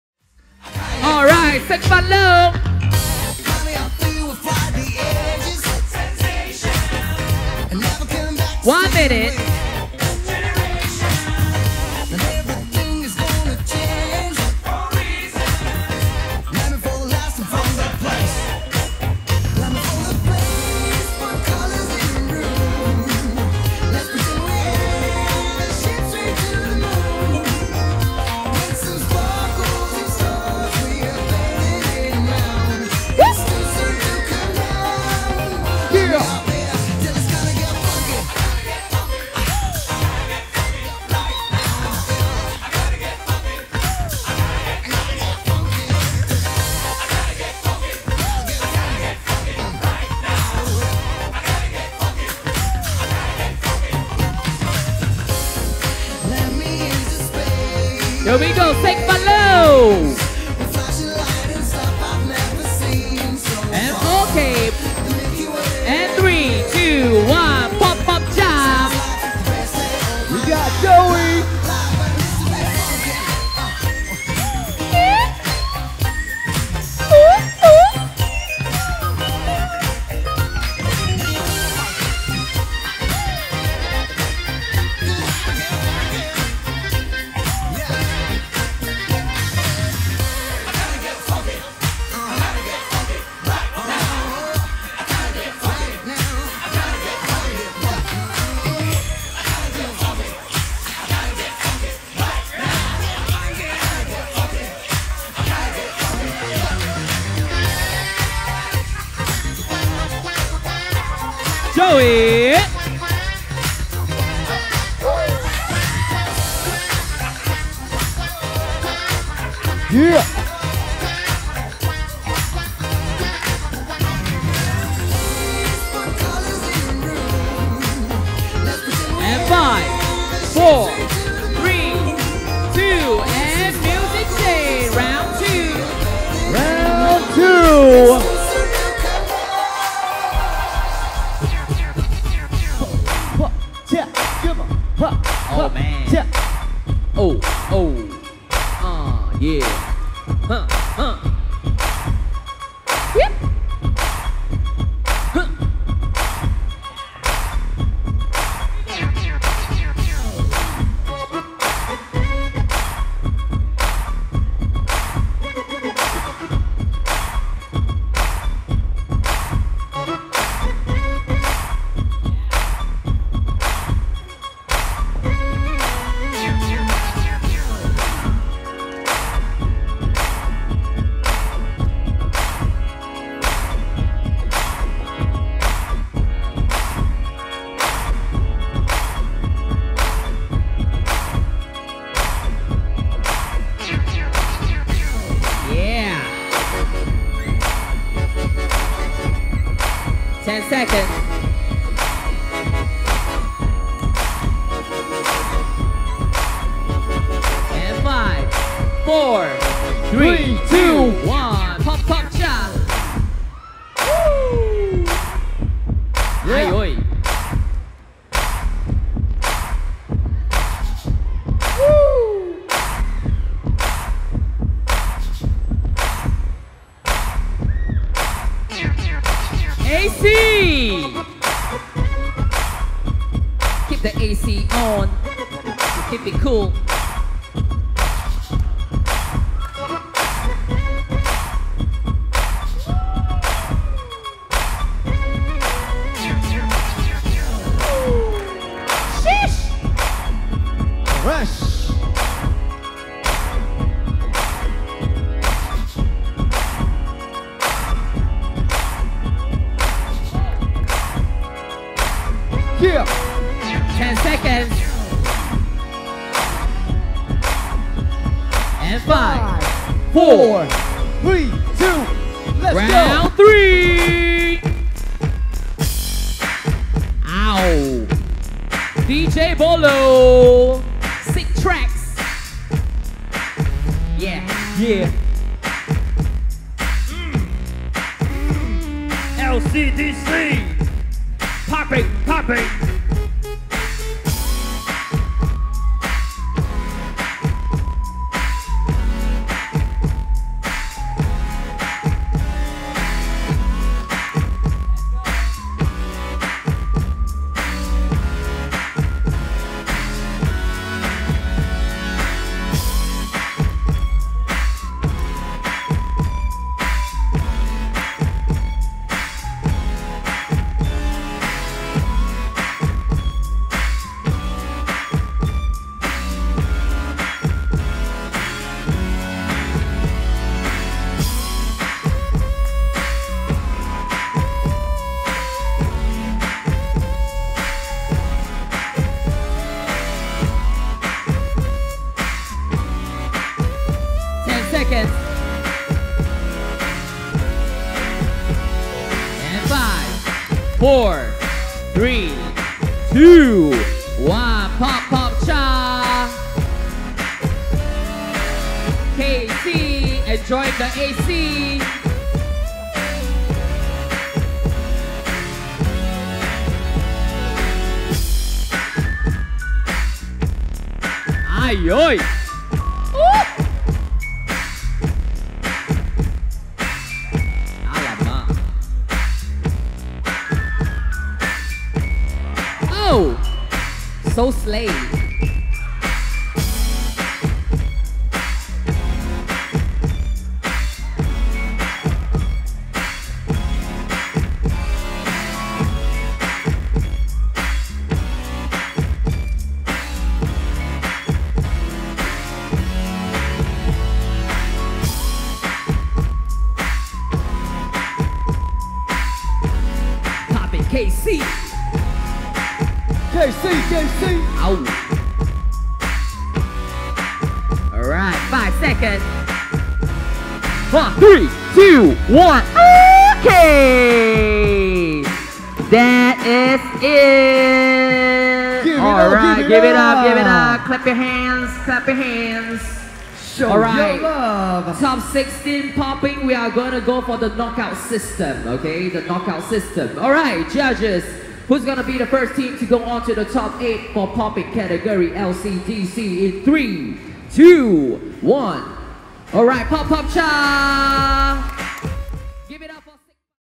Siek Van Loh vs Pop Pop Cha | Popping Top 16 | Lion City Dance Convention 2022
Group Dance Battle

Categories:
Open Styles 3v3 and Breaking 4v4

Qualifiers: 30 and 31 July
Finals (Top16) - 7 August

For the Open Styles 3v3 Qualifiers:
Represent Your Styles Edition

- Dancers of the same Dance Style will join in group of 3 and they will be judged respectively in their own genre.

- The Top 4 teams of each dance style be seeded into the TOP16 on Open Styles Finals.

- The Dance Styles are LOCKING, WAACKING, POPPING AND HIP HOP.

DJ Bolo
Emcee Jin

Judges:
Marcus Marzipan
Robin
Nic Ho

Filmed by Wen Jun Sam
Edited by TK
Thumbnail Design by Richard Prayoga